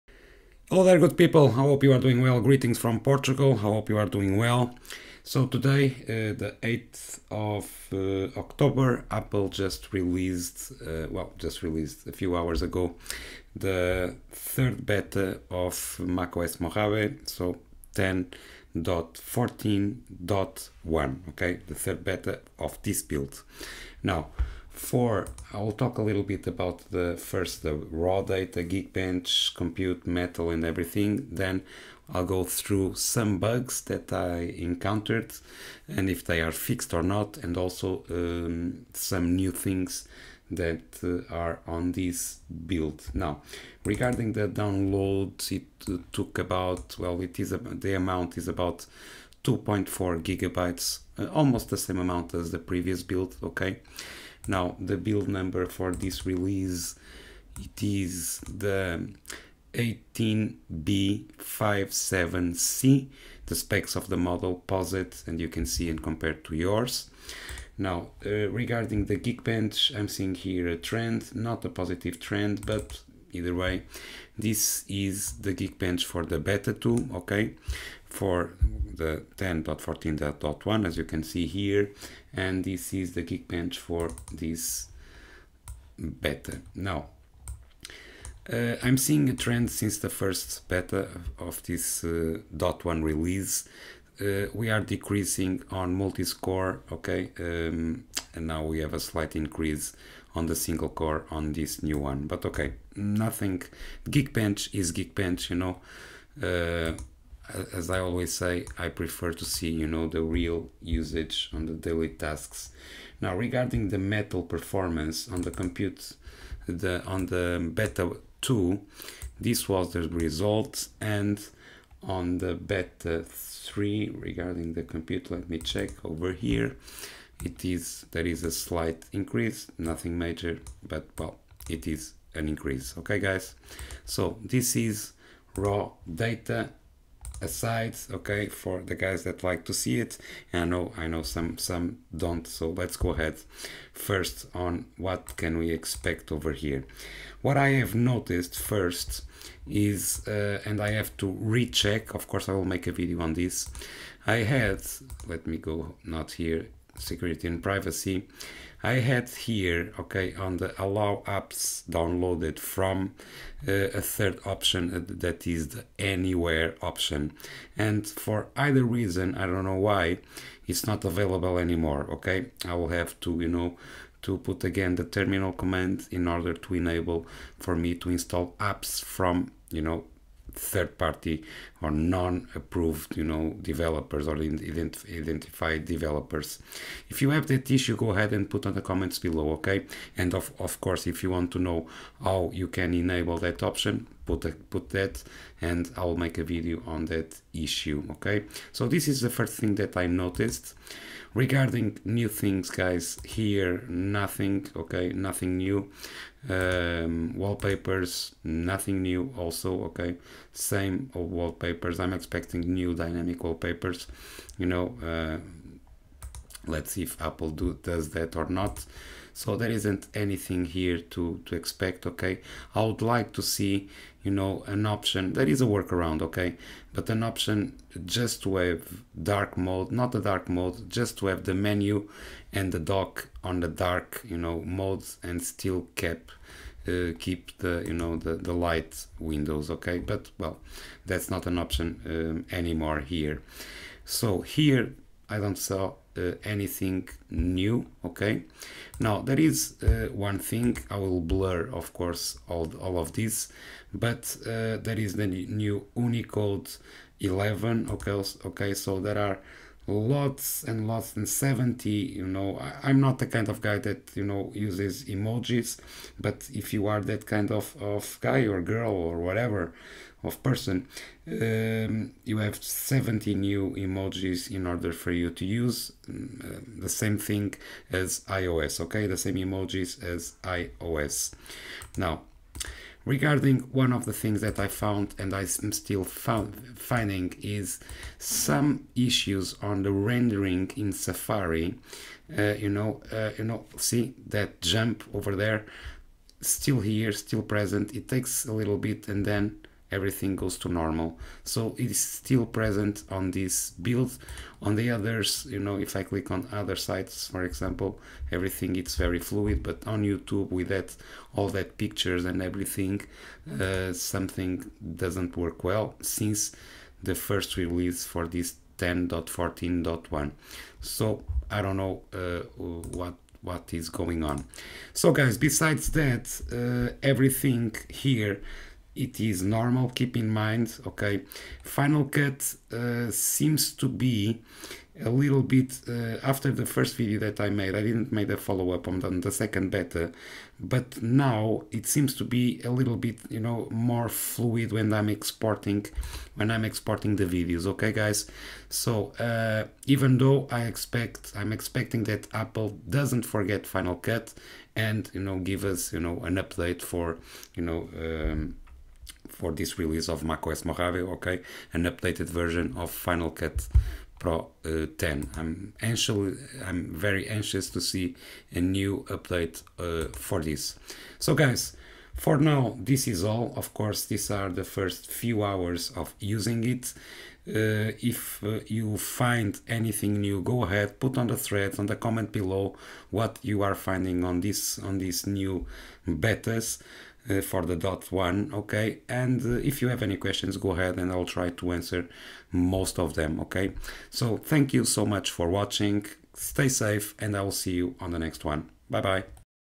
macOS Mojave 10 14 1 Beta 3 - First Look on Features and Bugs
macOS Mojave 10.14.1 Beta 3 released - What´s New